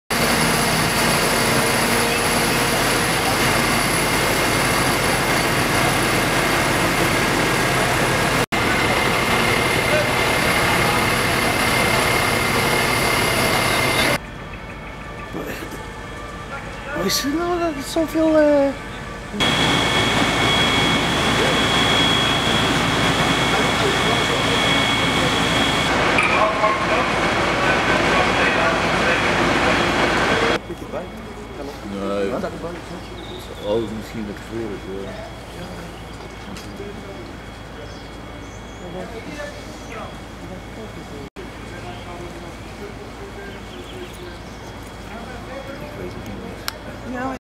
Afval in brand tussen twee panden in Molenstraat
Vrijdagavond heeft er een brand gewoed tussen twee panden aan de Molenstraat in Roosendaal. De brand ontstond in het afval in een ruimte van ongeveer 30 centimeter.